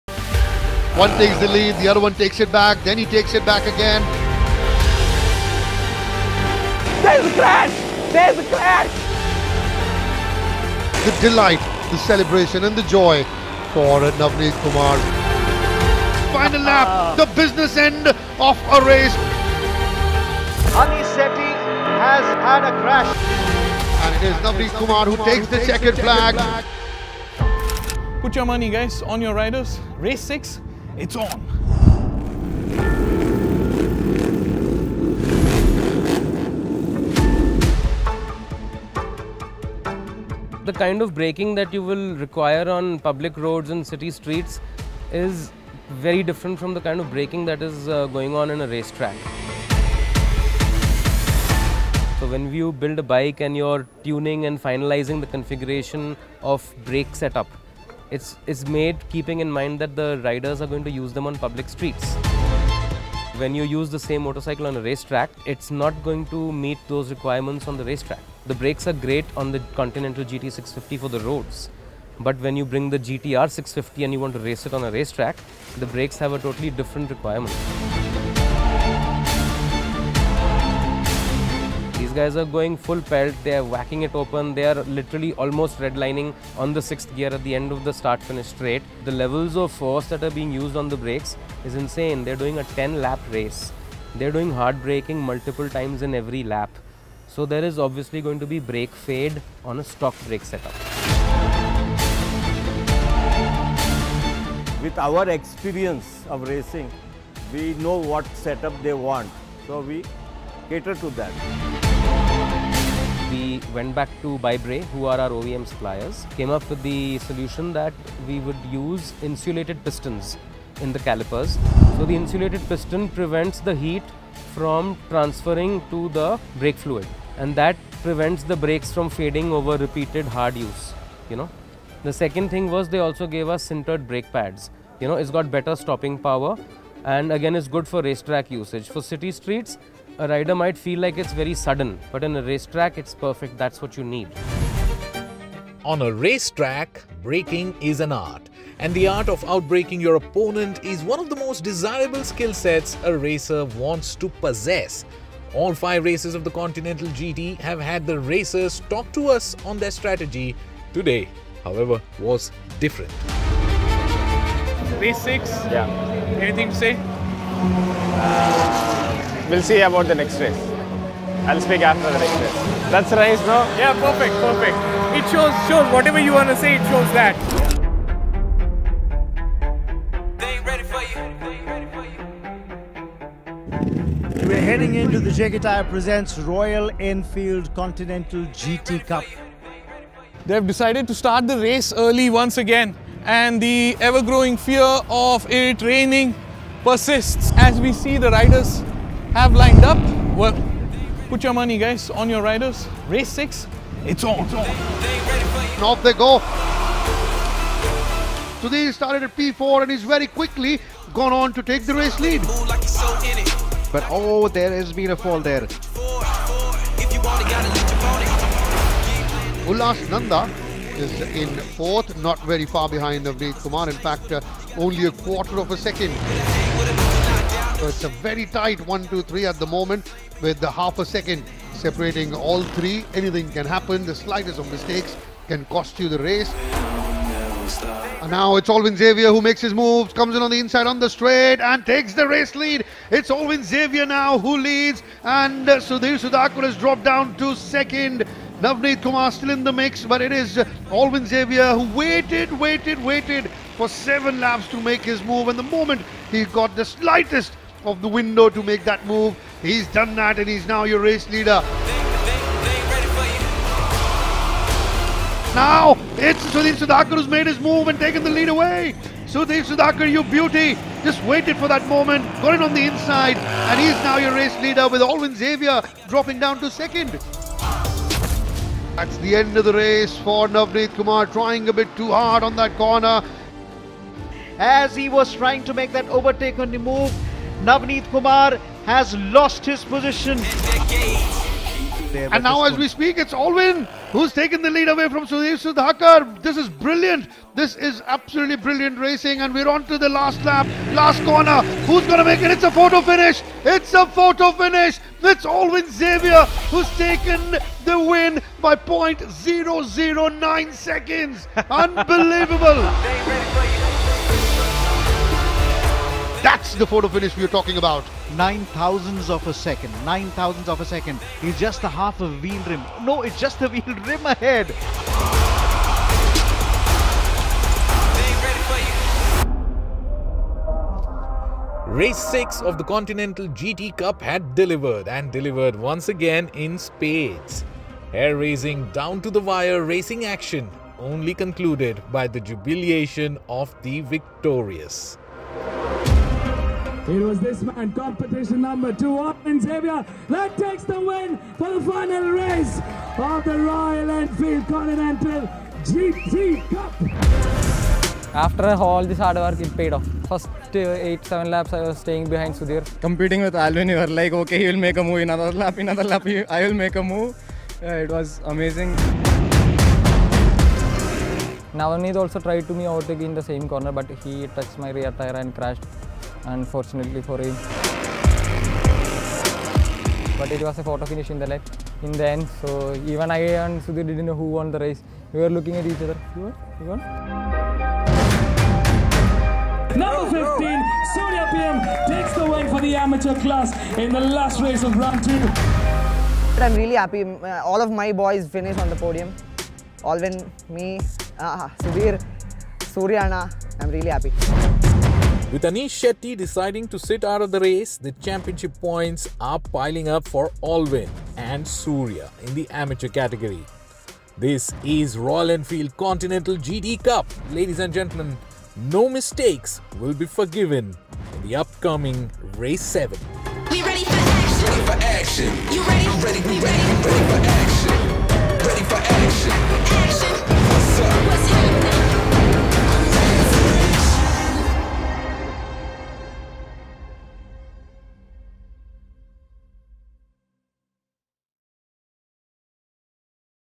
Royal Enfield Continental GT Cup S2 | The Art of Braking | Ep 06 | PowerDrift
The most important time is knowing when to stop, isn’t it? In this episode of the Royal Enfield Continental GT Cup S2, we break down the art of braking. How different is the use of brakes on public roads to that on a racetrack? We delve into the technical as well as practical aspect of how brakes and suspension function on the GT R-650 and how the riders get a grip of their bike, their lives and finally the championship!